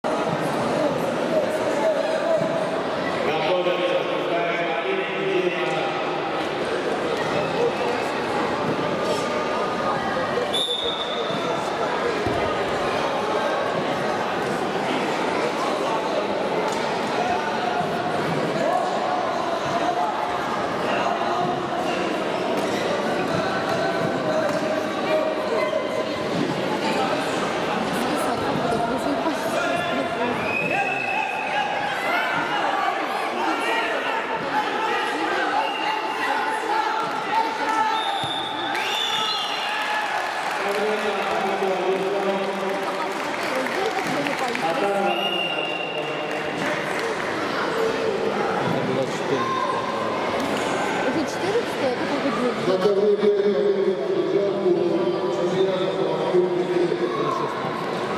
Мозгунов Ярослав (Сочи) - Татаров Алихан (Нальчик)
Весовая категория 30 кг
Открытый турнир Кавказских Минеральных Вод "Олимпийские надежды" по вольной борьбе среди юношей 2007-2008 и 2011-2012 г.р.
Кисловодск 23 января 2022.